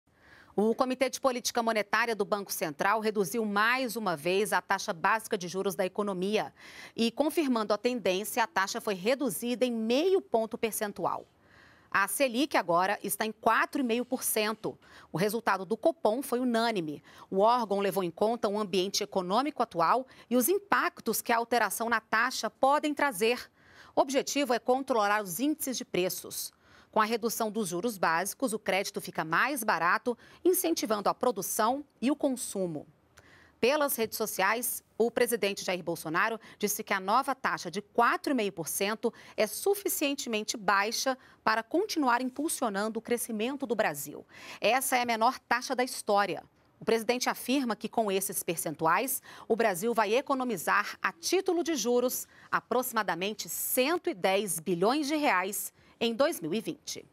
Copom baixa Selic para 4,5%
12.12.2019 - O Comitê de Política Monetária do Banco Central reduziu, mais uma vez, a taxa básica de juros da economia./ E confirmando a tendência, a taxa foi reduzida em meio ponto percentual.